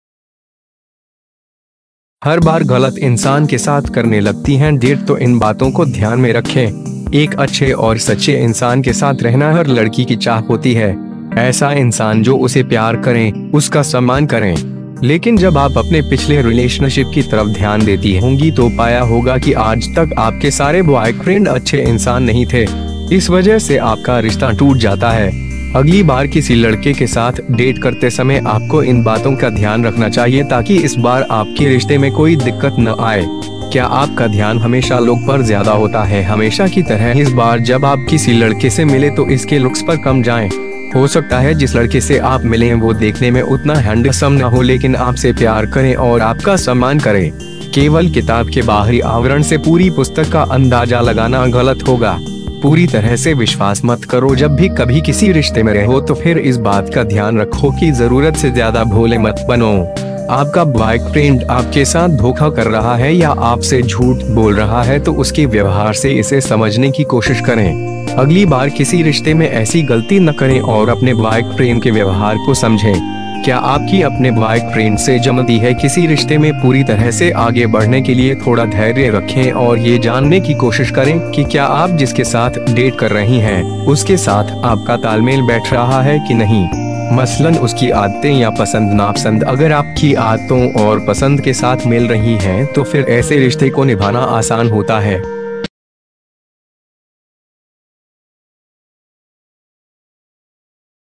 हर बार गलत इंसान के साथ करने लगती हैं डेट तो इन बातों को ध्यान में रखें
एक अच्छे और सच्चे इंसान के साथ रहना हर लड़की की चाह होती है। ऐसा इंसान जो उसे प्यार करें, उसका सम्मान करें। लेकिन जब आप अपने पिछले रिलेशनशिप की तरफ ध्यान देती होंगी तो पाया होगा कि आज तक आपके सारे ब्वायफ्रेंड अच्छे इंसान नहीं थे। इस वजह से आपका रिश्ता टूट जाता है। अगली बार किसी लड़के के साथ डेट करते समय आपको इन बातों का ध्यान रखना चाहिए ताकि इस बार आपके रिश्ते में कोई दिक्कत न आए।
क्या आपका ध्यान हमेशा लुक पर ज्यादा होता है
हमेशा की तरह इस बार जब आप किसी लड़के से मिले तो इसके लुक्स पर कम जाएं। हो सकता है जिस लड़के से आप मिलें वो देखने में उतना हैंडसम न हो लेकिन आपसे प्यार करें और आपका सम्मान करे। केवल किताब के बाहरी आवरण से पूरी पुस्तक का अंदाजा लगाना गलत होगा।

पूरी तरह से विश्वास मत करो
जब भी कभी किसी रिश्ते में रहो तो फिर इस बात का ध्यान रखो कि जरूरत से ज्यादा भोले मत बनो। आपका ब्वायफ्रेंड आपके साथ धोखा कर रहा है या आपसे झूठ बोल रहा है तो उसके व्यवहार से इसे समझने की कोशिश करें। अगली बार किसी रिश्ते में ऐसी गलती न करें और अपने ब्वायफ्रेंड के व्यवहार को समझें।

क्या आपकी अपने ब्वायफ्रेंड से जमती है
किसी रिश्ते में पूरी तरह से आगे बढ़ने के लिए थोड़ा धैर्य रखें और ये जानने की कोशिश करें कि क्या आप जिसके साथ डेट कर रहीं हैं, उसके साथ आपका तालमेल बैठ रहा है कि नहीं। मसलन उसकी आदतें या पसंद नापसंद अगर आपकी आदतों और पसंद के साथ मिल रहीं हैं तो फिर ऐसे रिश्ते को निभाना आसान होता है।